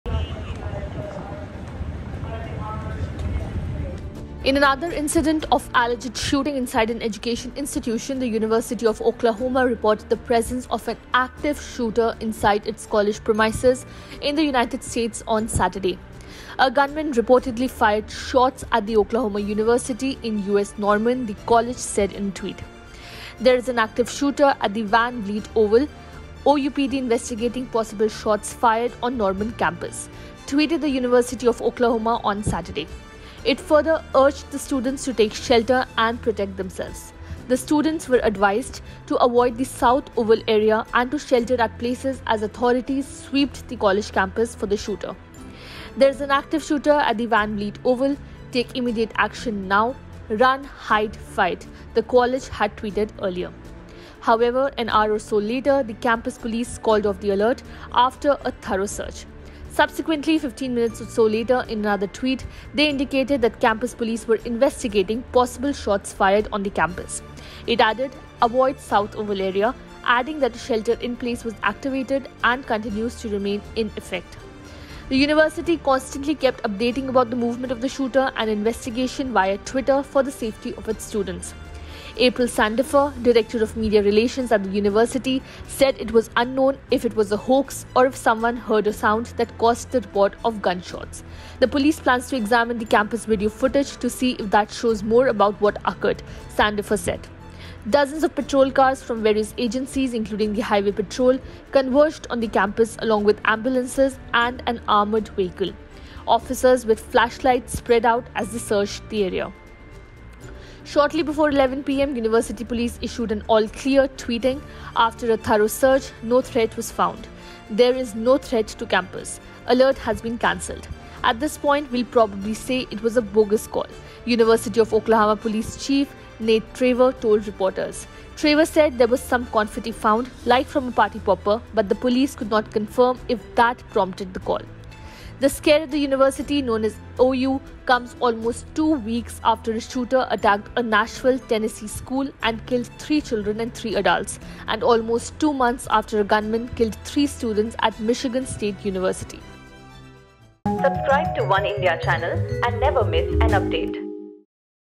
US University reports of an active shooter, asks students to take shelter | Oneindia News
In another incident of shooting inside an education institution, the University of Oklahoma reported the presence of an active shooter inside its college premises in the United States on Saturday. A gunman reportedly fired shots at the University of Oklahoma in US' Norman, the college said in a tweet.